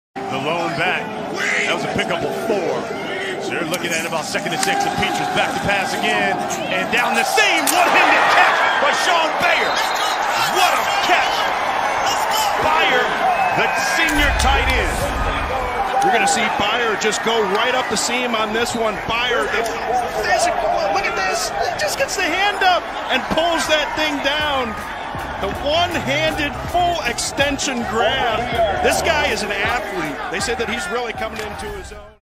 Iowa tight end Shaun Beyer makes a ridiculous one-handed catch in the first half against Nebraska
Iowa Hawkeyes tight end Shaun Beyer makes a ridiculous one-handed catch in the first half against Nebraska.